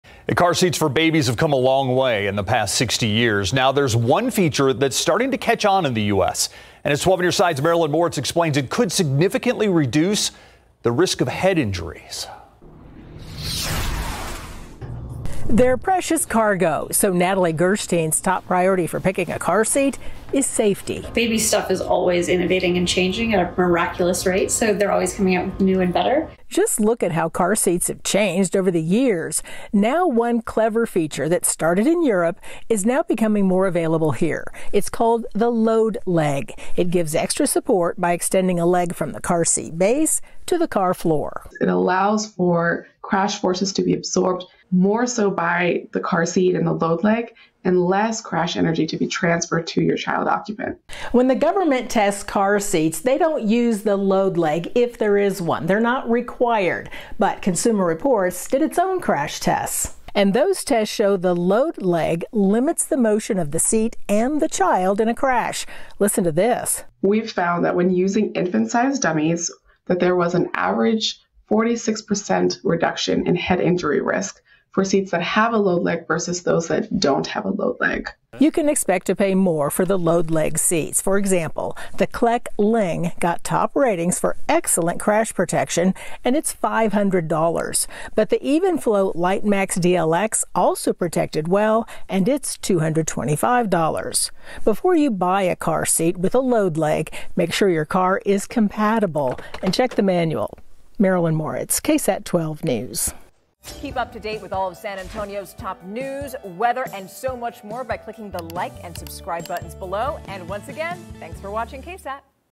‘Load leg’ can improve infant car seat safety
Children’s car seats have come a long way in the past 60 years, and Consumer Reports’ testing shows that progress is continuing, with the the emergence of a feature called the load leg.